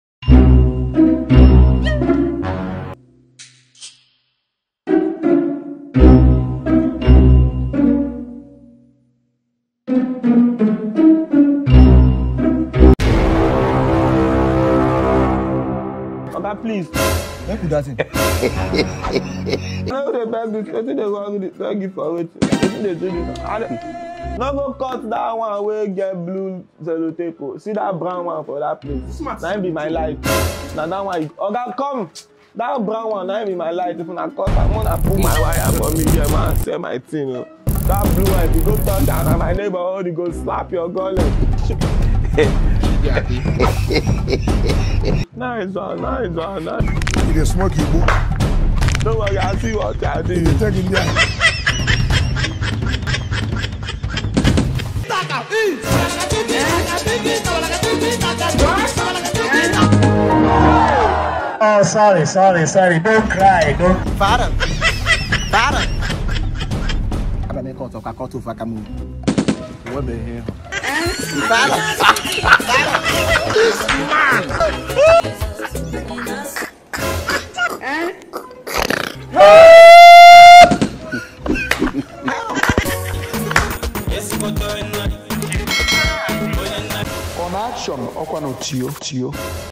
funnybros cut brainjotter light trouble (nepa) - latest Nigeria comedy skit
latest Nigeria comedy skit starring brain jotter and funnybroscomedy